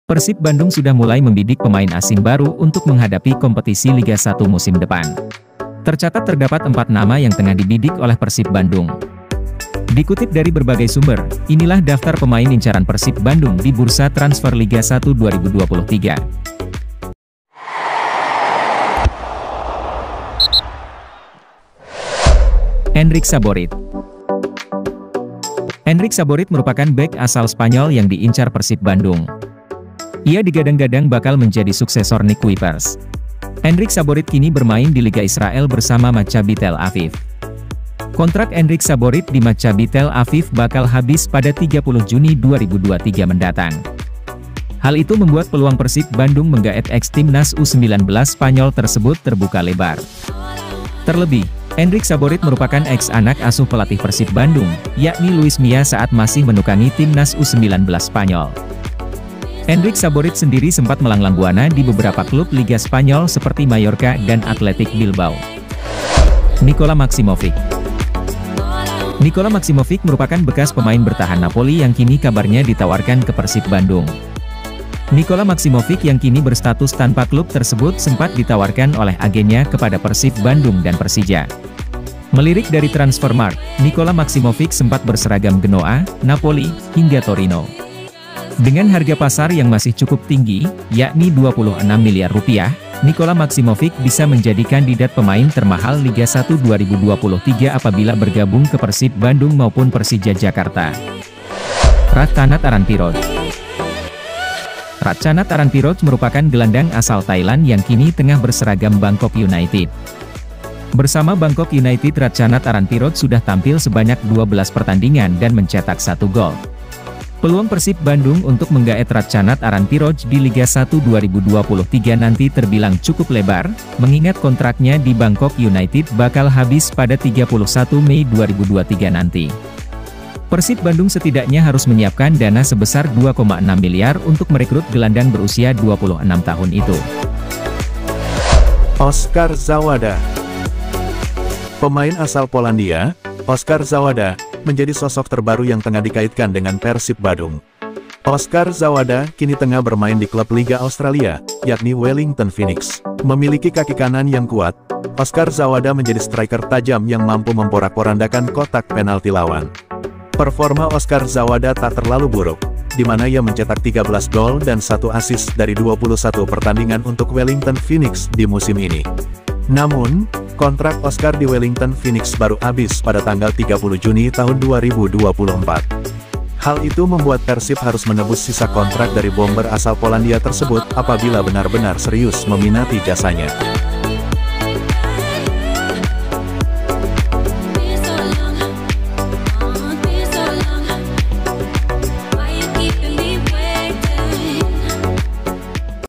4 Pemain Asing Incaran Persib Bandung | Update Transfer Liga 1 2023
Bursa transfer Liga 1 2023 memang belum resmi dibuka. Namun, Persib Bandung sudah mulai membidik pemain asing baru untuk menghadapi kompetisi Liga 1 musim depan.

Tercatat terdapat 4 nama yang tengah dibidik oleh Persib Bandung untuk menghadapi musim mendatang. Dikutip dari berbagai sumber inilah daftar pemain incaran Persib Bandung di bursa transfer Liga 1 2023.

Enric Saborit.
Enric Saborit kini bermain di Liga Israel bersama Tel Aviv Maccabi. kontrak Enric Saborit di Maccabi Tel Aviv bakal habis pada 30 Juni 2023 mendatang.

Nikola Maksimovic.
Nikola Maksimovic merupakan eks pemain bertahan Napoli yang kini kabarnya ditawarkan ke Persib Bandung dan Persija Jakarta. Nikola Maksimovic sempat berseragam Genoa, Napoli, hingga Torino.

Ratchanat Aranpiroj.
Ratchanat Aranpiroj/Arunyapairot merupakan gelandang asal Thailand yang kini tengah berseragam Bangkok United di Liga Thailand.

Oskar Zawada.
Oskar Zawada sendiri kini tengah bermain di klub Liga Australia, yakni Wellington Phoenix.

+Tags.